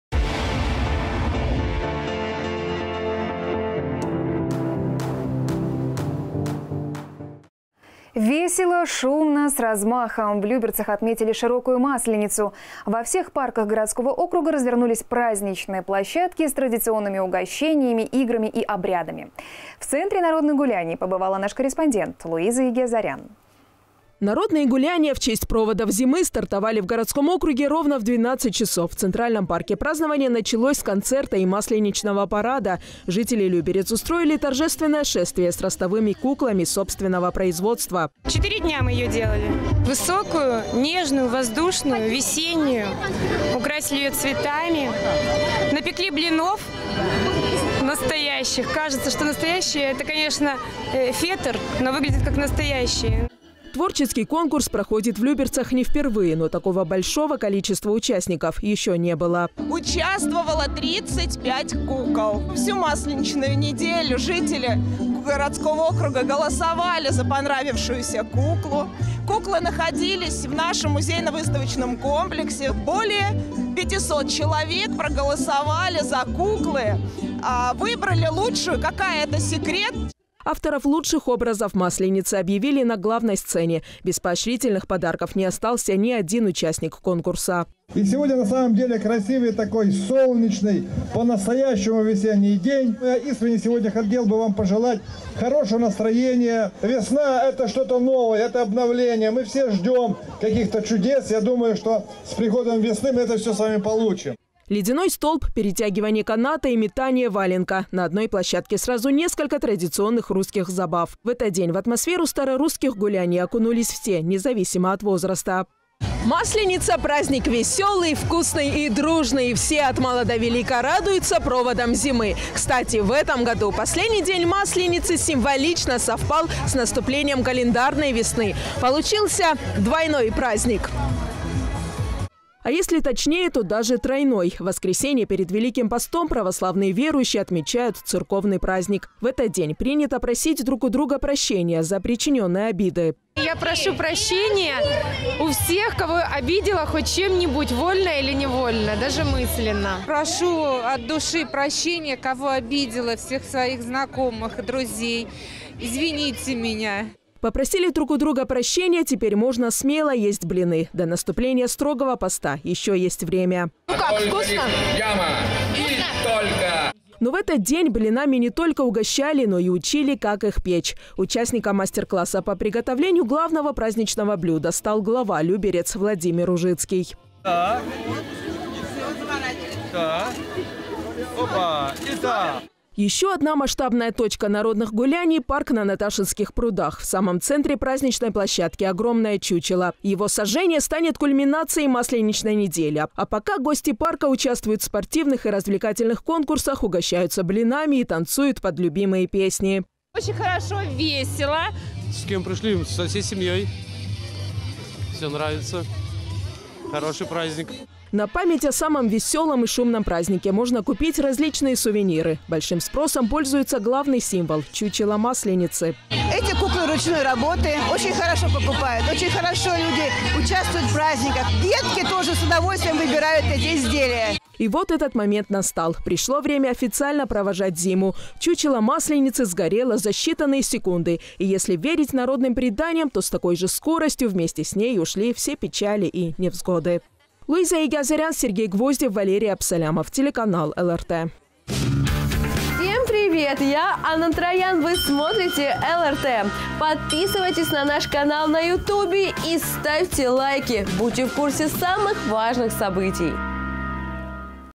Масленица идет, блины да мед несёт!
Весело, шумно, с размахом! В Люберцах отметили Широкую Масленицу. Во всех парках городского округа развернулись праздничные площадки с традиционными угощениями, играми и обрядами. В центре народных гуляний побывала Луиза Егиазарян.

Народные гуляния в честь проводов зимы стартовали в городском округе ровно в 12 часов. В Центральном парке празднование началось с концерта и масленичного парада. Жители Люберец устроили торжественное шествие с ростовыми  куклами собственного производства.

Ирина Корсарь, воспитатель детского сада No28 г.о. Люберцы:
"4 дня мы ее делали высокую, нежную, воздушную, весеннюю, украсили ее цветами, напекли блинов, кажется, что настоящие, это, конечно, эффект, но выглядят, как настоящие".

Творческий конкурс проходит в Люберцах не впервые, но такого большого количества участников еще не было.

Виктория Фетисова, заведующая отделом развития музейно-выставочного комплекса г.о. Люберцы:
"Участвовало 35 кукол. Всю масленичную неделю жители городского округа голосовали за понравившуюся куклу, куклы находились в нашем музейно-выставочном комплексе 01.10 СКЛ 01.14 Более пятисот человек проголосовали за куклы, выбрали лучшую".

Авторов лучших образов  Масленицы объявили на главной сцене. Без поощрительных подарков не остался ни один участник конкурса.

Владимир Ружицкий, глава г.о. Люберцы:
"Сегодня, на самом деле, красивый такой, солнечный, по-настоящему весенний день. Я искренне хотел бы вам пожелать хорошего настроения. Весна – это что-то новое, это обновление, мы все ждем каких-то чудес, я думаю, что с приходом весны мы все это с вами получим".

Ледяной столб, перетягивание каната и метание валенка. На одной площадке сразу несколько традиционных русских забав. В этот день в атмосферу  старорусских гуляний окунулись все, независимо от возраста.

Луиза Егиазарян, корреспондент:
"Масленица-праздник весёлый, вкусный и дружный. Все, от мала до велика радуются проводам зимы. Кстати, в этом году последний день Масленицы символично совпал с наступлением календарной весны. Получился двойной праздник".

А если точнее, то даже тройной. В воскресенье перед Великим постом православные верующие отмечают церковный праздник. В этот день принято просить друг у друга прощения за причинённые обиды.

"Я прошу прощения у всех, кого обидела хоть чем-нибудь, вольно или невольно, даже мысленно".
" Прошу от души прощения всех, кого обидела. Всех своих знакомых, друзей, извините меня".

Попросили друг у друга прощения, теперь можно смело есть блины. До наступления строгого поста ещё есть время.

Но в этот день блинами не только угощали, но и учили, как их печь. Участником мастер-класс по приготовлению главного праздничного блюда стал глава Люберец Владимир Ружицкий.

Ещё одна масштабная точка народных гуляний - парк на Наташинских прудах. В самом центре праздничной площадки - огромное чучело. Его сожжение станет кульминацией масленичной недели. А пока гости парка участвуют в спортивных и развлекательных конкурсах, угощаются блинами и танцуют под любимые песни.

"Очень хорошо, весело".

"С кем пришли? Со всей семьей. Всем нравится, хороший праздник"

На память о самом веселом и шумном празднике можно купить различные сувениры. Большим спросом пользуется главный символ – чучело Масленицы.

"Эти куклы ручной работы. Очень хорошо покупают, очень хорошо люди участвуют в празднике, детки тоже с удовольствием выбирают эти изделия".

И вот этот момент настал. Пришло время официально провожать зиму. Чучело Масленицы сгорело за считанные секунды. И, если верить народным приданиям, то с такой же скоростью вместе с ней ушли все печали и невзгоды.

Луиза Егиазарян. Сергей Гвоздев. Валерий Абсалямов.